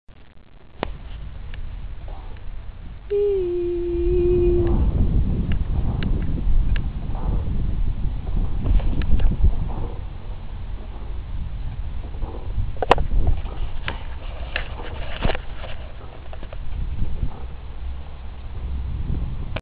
Adrian ut og disser sammen med meg.Han villa absolutt ha på seg disse brillene.;)
Adrian ut og disser sammem med mormor.Han villa absolutt ha på seg disse brillene.;)